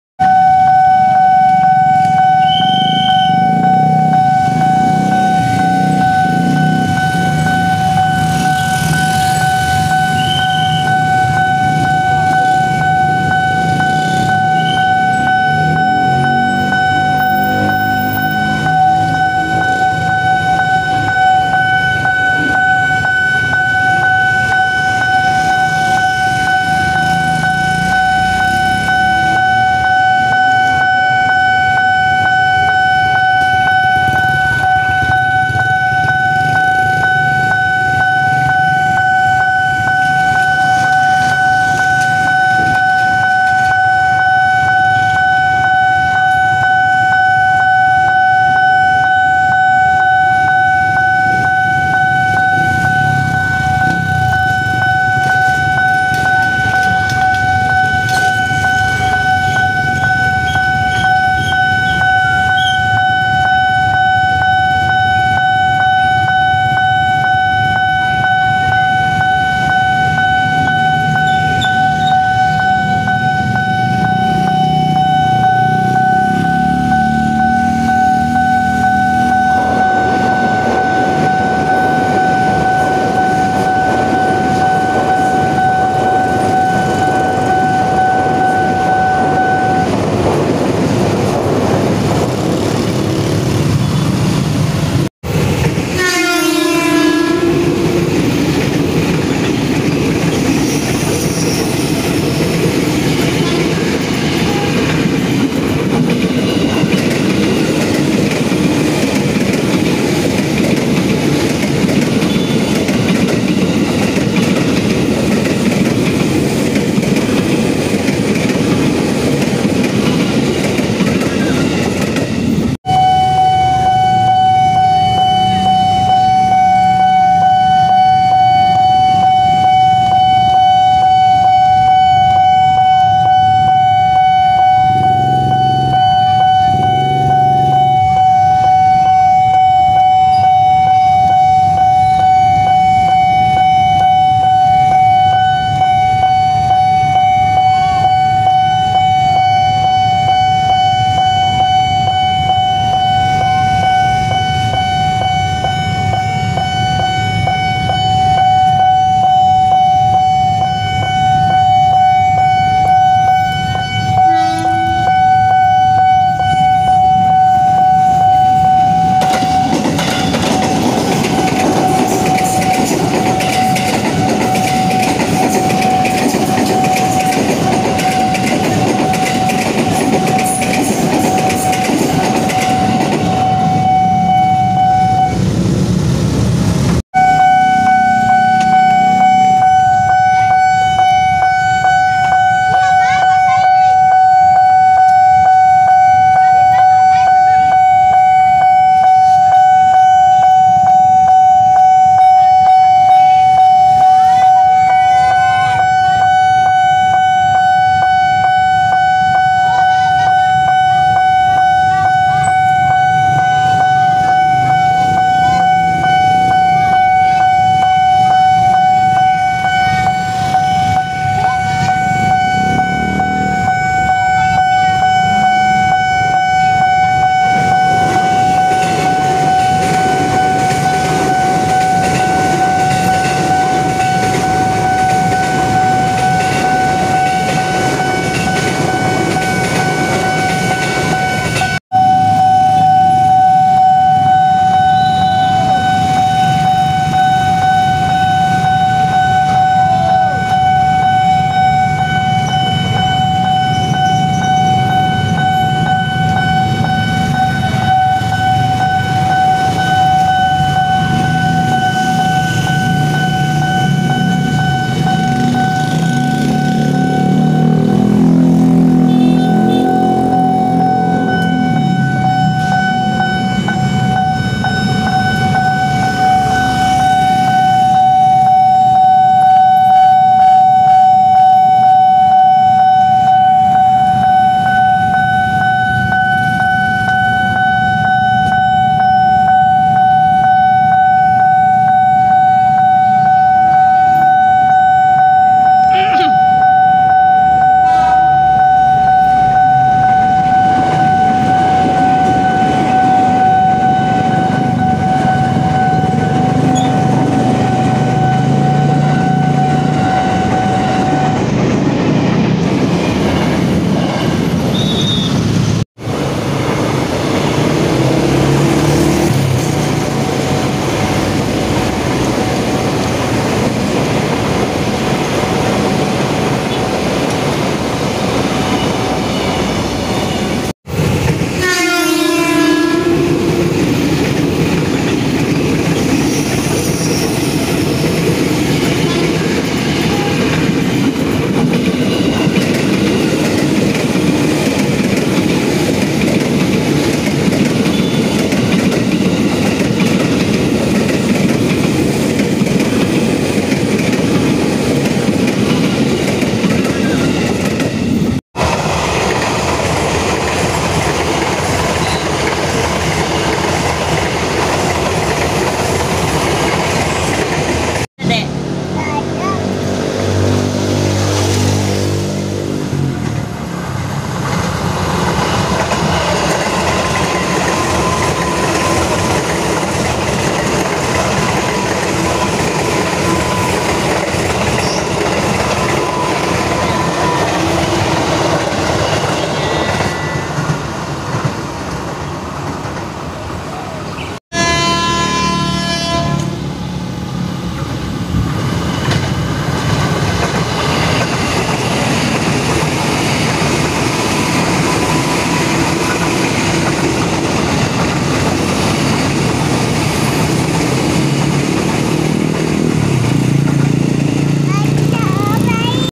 Perlintasan Kereta Api KRL Kebon Pedes Tanah Sareal - Railway Crossing Indonesia
Perlintasan Kereta Api KRL Bogor - Railway Crossing Indonesia

NGERI!!
Akhirnya nemu Kereta api KRL di Perlintasan Bogor yaitu daerah Kebum Pedes - Pondok Rumput dekat pemberhentian Stasiun Bogor Jawa Barat Railway Crossing Bogor.